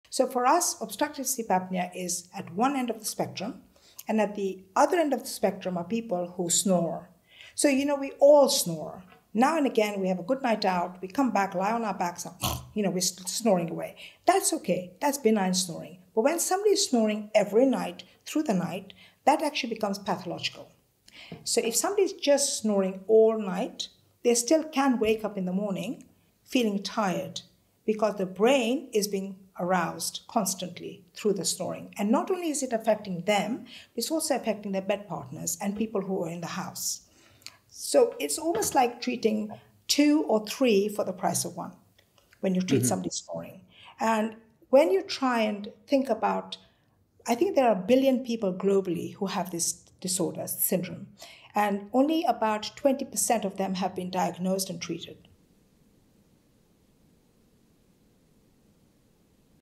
Obstructive Sleep Apnea
If you want to learn about the impact of snoring and sleep apnoea and the role of Dentists, check out PDP139 on wherever you usually get your podcasts, on YouTube or for a brucey bonus,

Thank you Dr Aditi Desai for sharing your energy and your expertise!